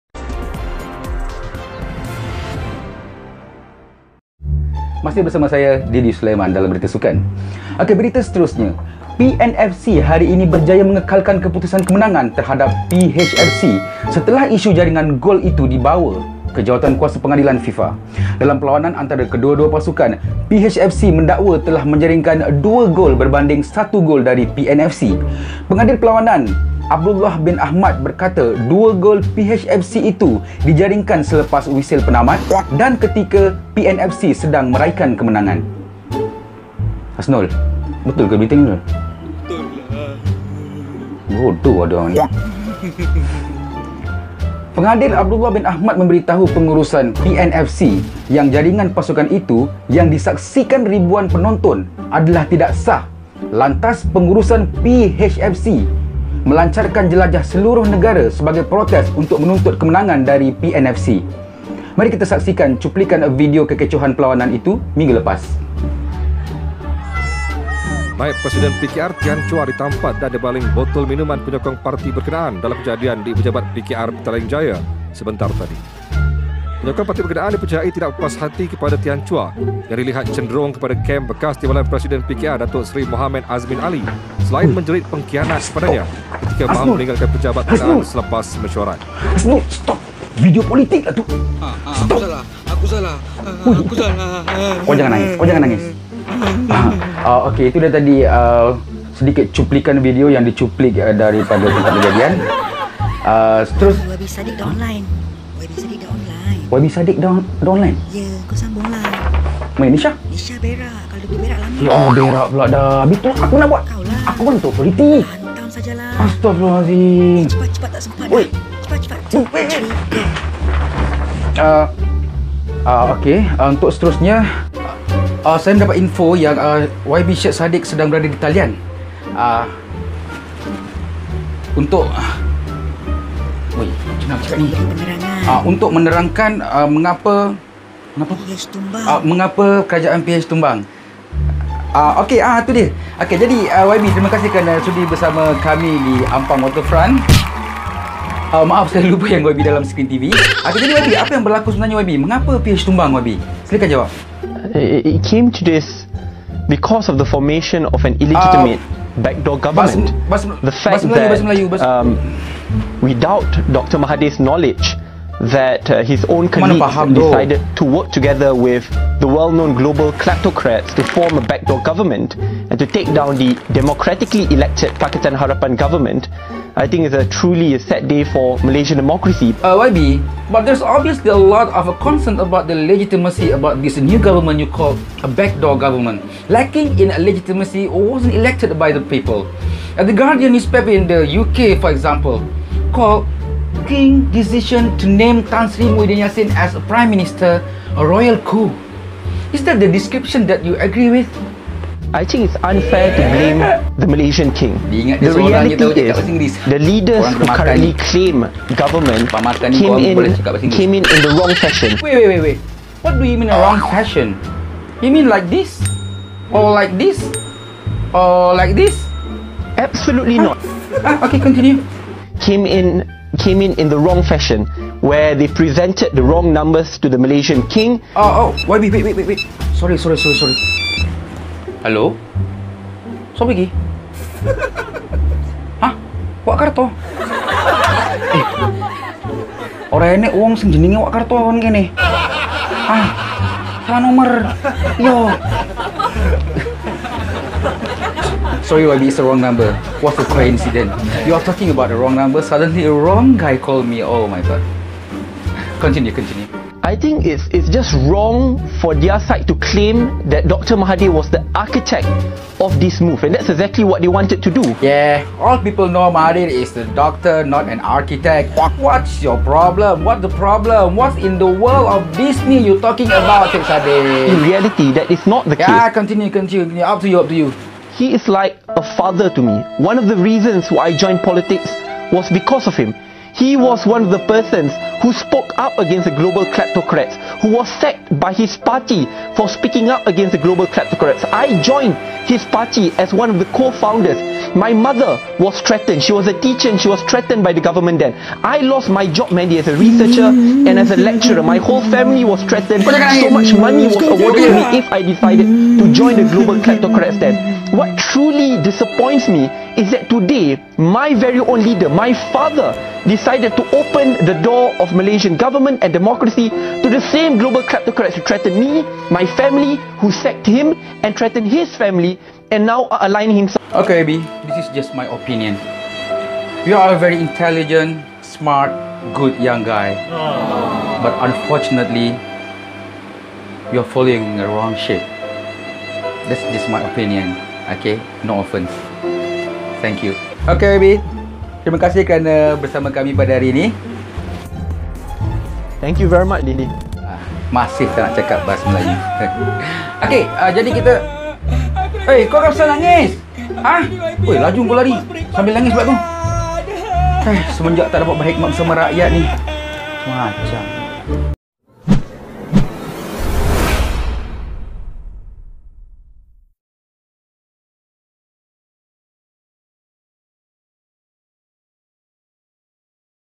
(PARODI) #5 Upfront Al Jazeera interview with YB Syed Saddiq - PH F.C vc PN F.C
Tak sengaja interview syed saddiq hari ni. Hari ni YB Hasnul bekas Exco Perak jadi krew penerbitan. Hahaha. Sekali lagi diingatkan ini hanyalah Parodi. Dilarang sentap.